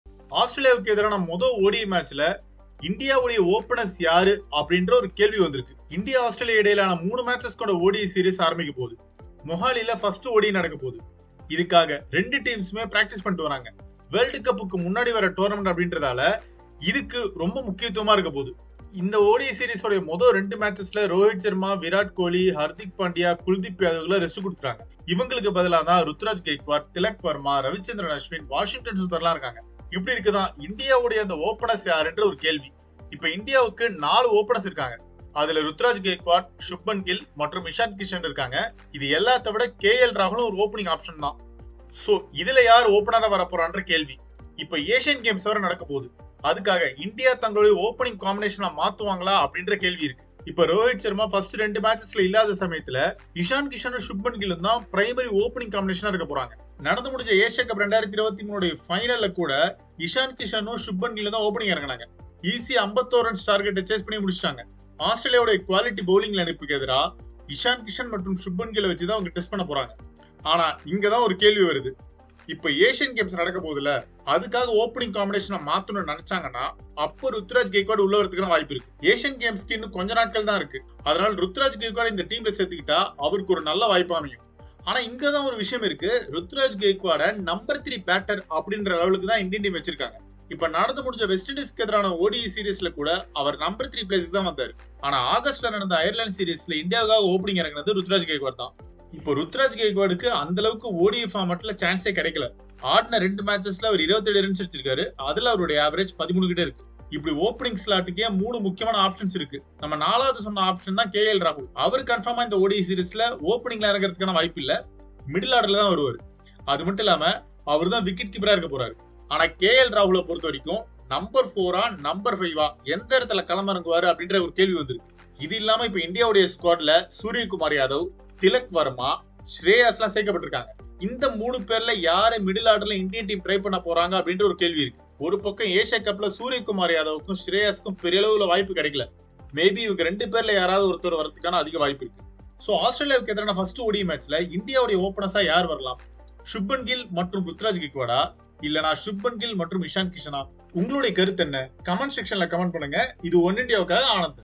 IND vs AUS 1st ODI: போட்டியில் Ruturaj vs Ishan! Gill-உடன் Opening Pair யார்? | Oneindia Howzat
ஆஸ்திரேலிய அணிக்கு எதிரான முதல் ஒருநாள் போட்டியில் இஷான் கிஷன் மற்றும் ருதுராஜ் கெய்க்வாட் இருவரில் யார் தொடக்க வீரராக களமிறங்குவார்கள் என்ற கேள்வி எழுந்துள்ளது.

                ~PR.55~ED.72~HT.73~